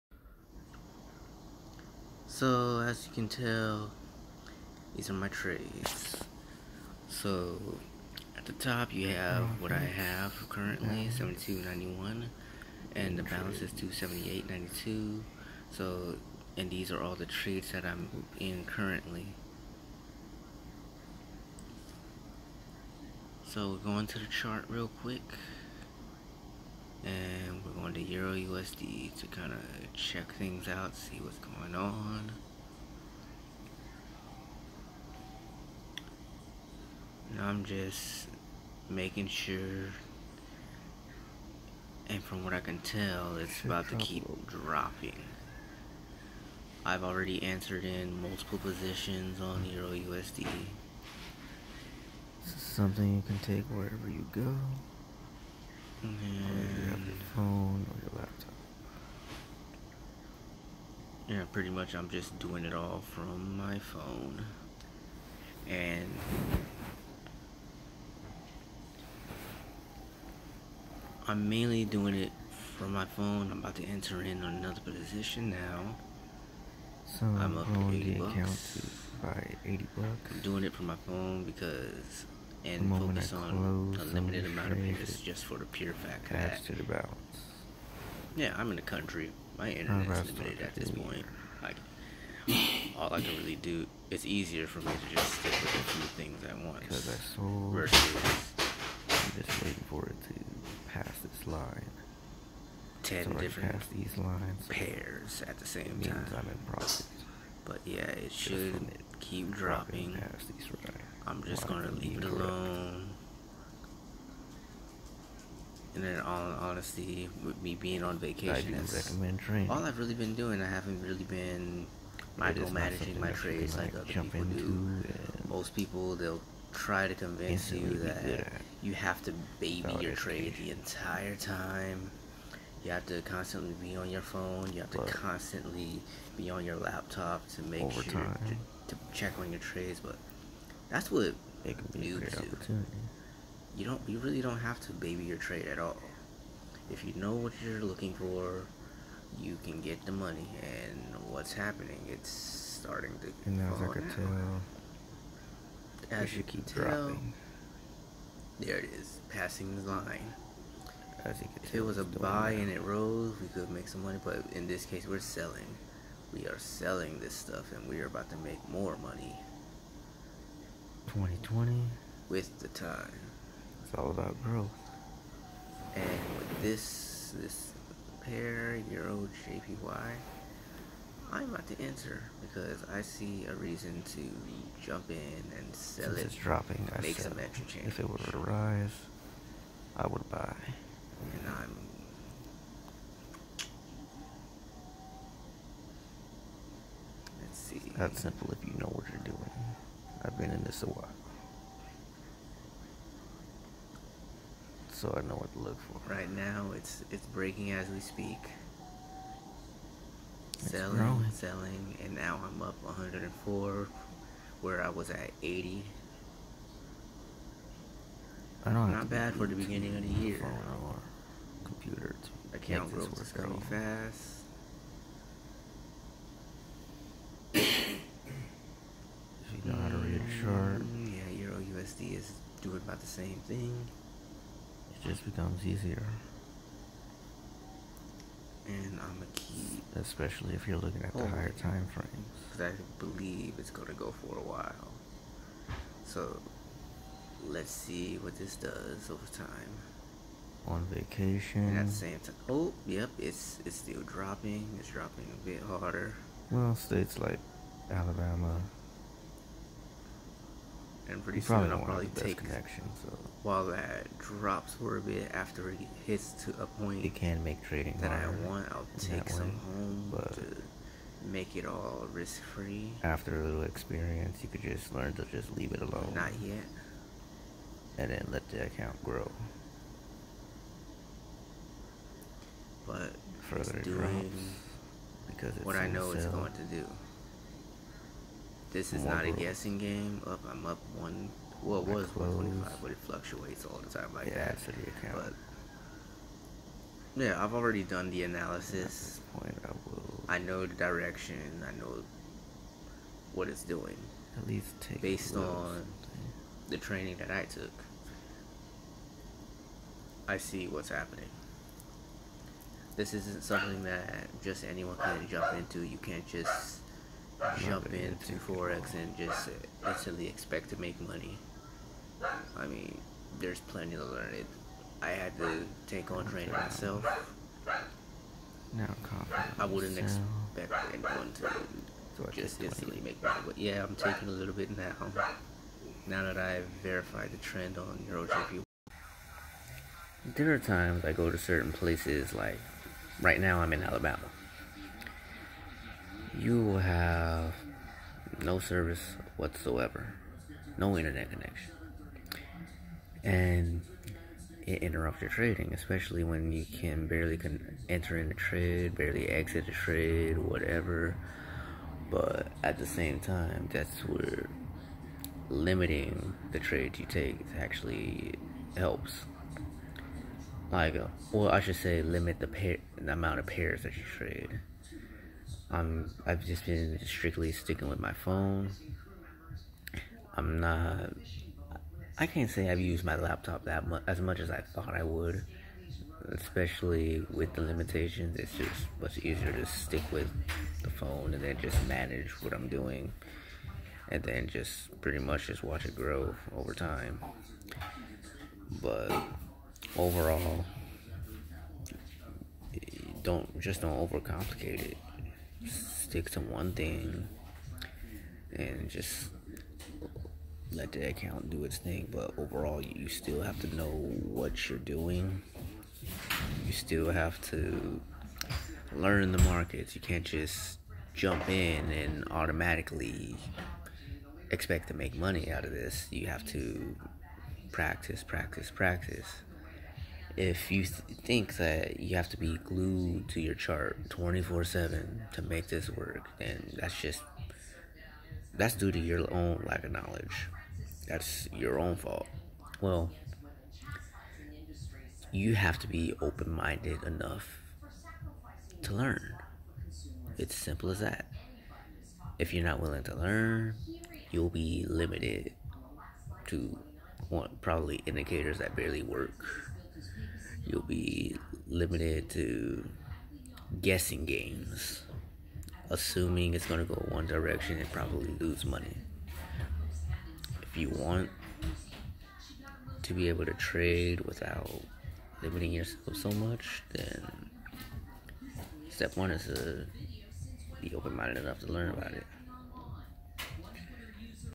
My Life As A Forex Trader: First Live FOREX Trade of 2020
My life as a forex trader began in the year 2015 and ever since it has been a long life changing journey full of growth. I love trading Forex and to start this year right here is my first live forex trade of the year 2020. Trading with limited internet connection and how to overcome it